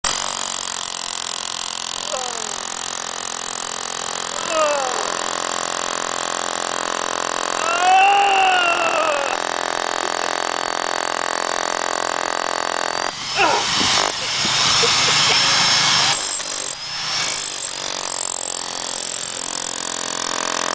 Jigger Vibe!.MPG
Steve taking the boredome out of digging holes...